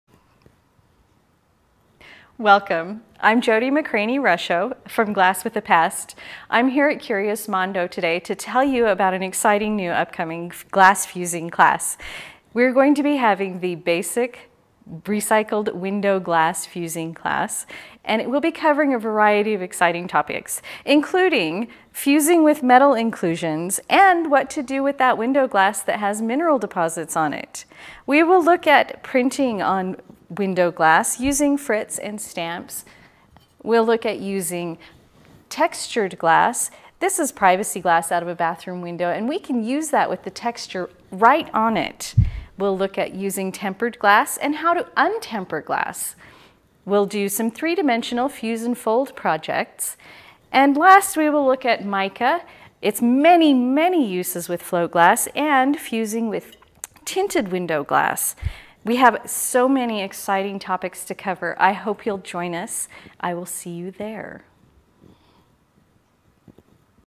Fusing with Recycled Window (Float) Glass
Float glass is a blank canvas just waiting for your creativity! Learn how to identify different types of window glass and fuse with them. We will learn to identify the tin side of float glass, and what that means for you. Explore how to add metals and other inclusions to your float glass for more complicated designs. Discover new ways to use glass paints and enamels to broaden your fusing knowledge. Delve into glass additions including decals, dichro slides and extracts, lusters and other amazing products.